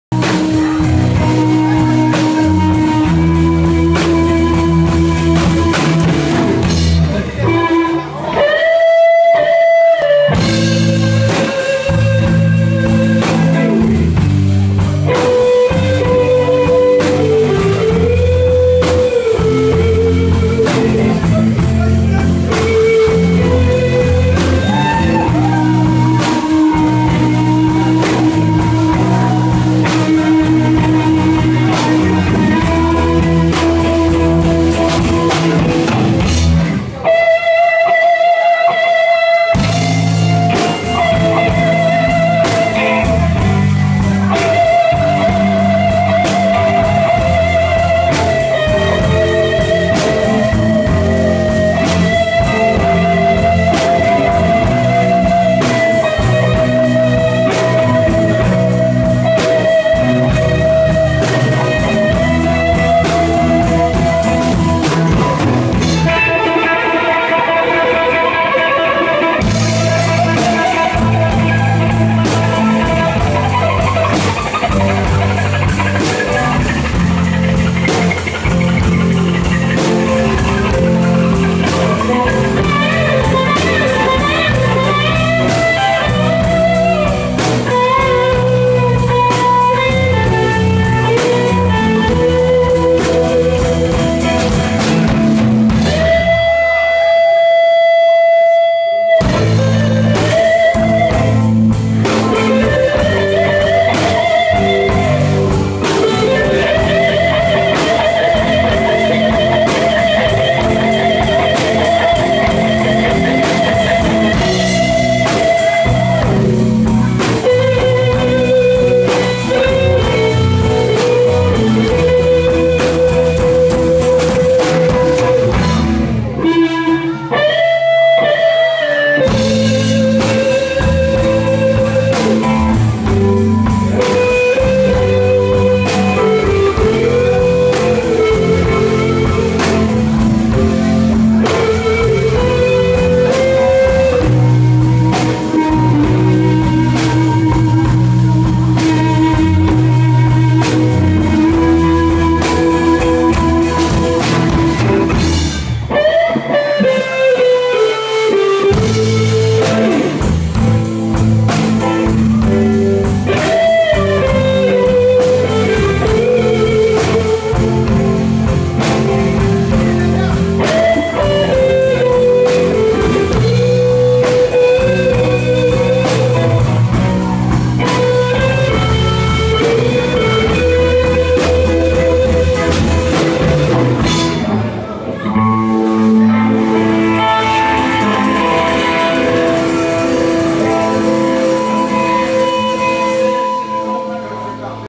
Pike river blues band. Hotel lambton le 6 decembre
Un super de bon groupe rock annee 60's 70's